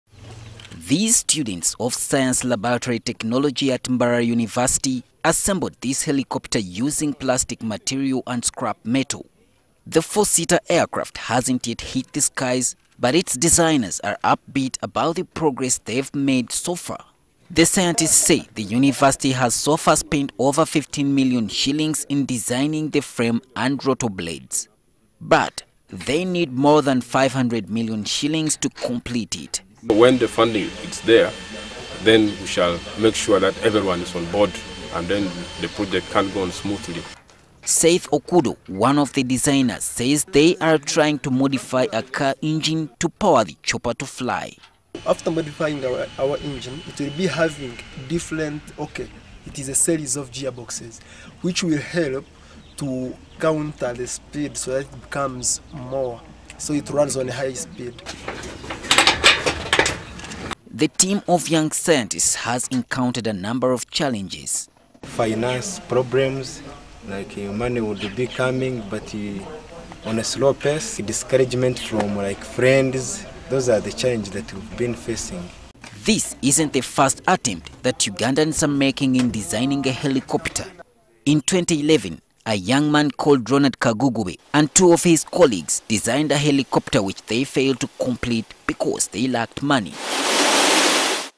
MUST students make helicopter
Four students of Mbarara University of Science and Technology have assembled a helicop-ter using scrap metal at the university campus in western Uganda